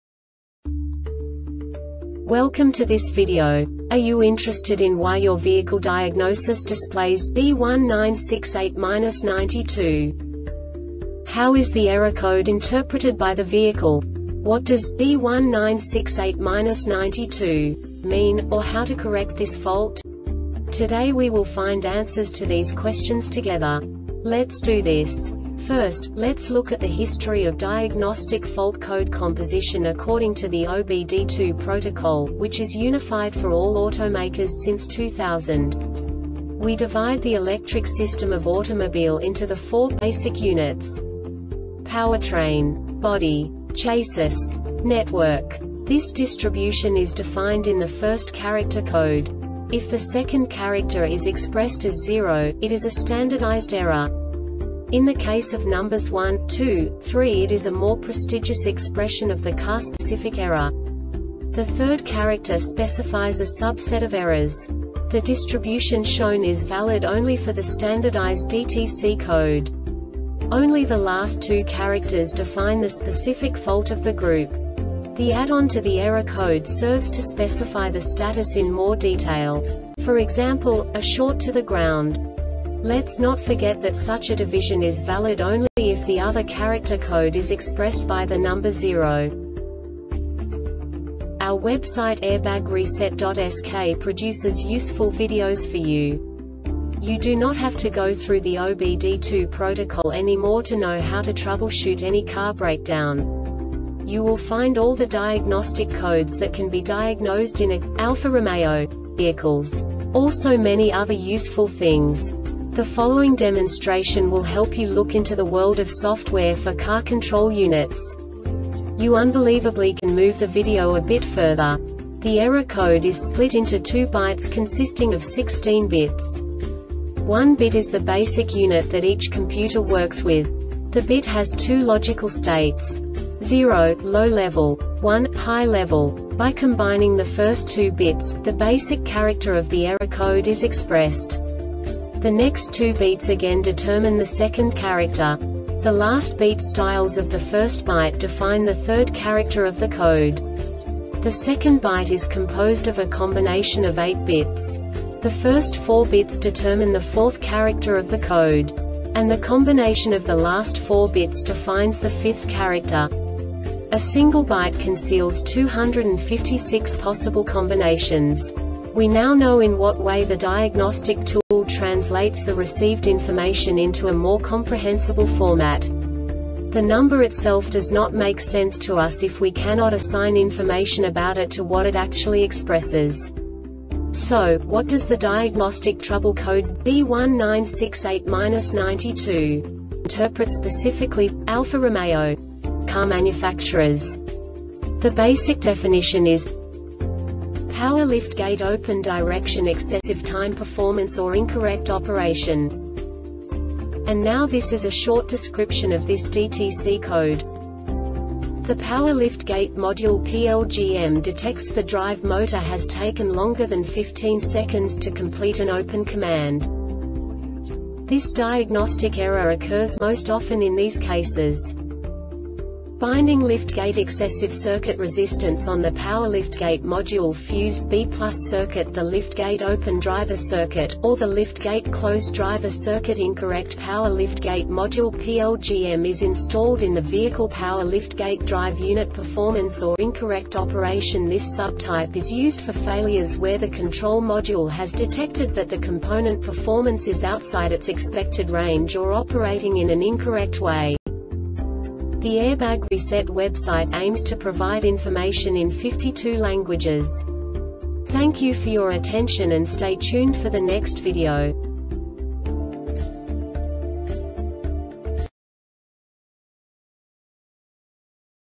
DTC Alfa-Romeo B1968-92 Short Explanation
Contents:
0:21 Basic DTC analysis according to OBD2 protocol standard.
1:48 Insight into programming
2:58 A diagnostically understandable description of the Diagnostic Trouble Code
3:05 Basic error definition

Make:
Alfa Romeo
Code:
B1968-92
Definition:
Power Liftgate Open Direction Excessive Time-Performance Or Incorrect Operation
Description:
The Power Liftgate Module (PLGM) detects the drive motor has taken longer than 15 seconds to complete an open command.
Cause:
Binding Liftgate
Excessive Circuit Resistance On The Power Liftgate Module Fused B(+) Circuit
The Liftgate Open Driver Circuit, Or The Liftgate Close Driver Circuit
Incorrect Power Liftgate Module (Plgm) Is Installed In The Vehicle
Power Liftgate Drive Unit
Failure Type: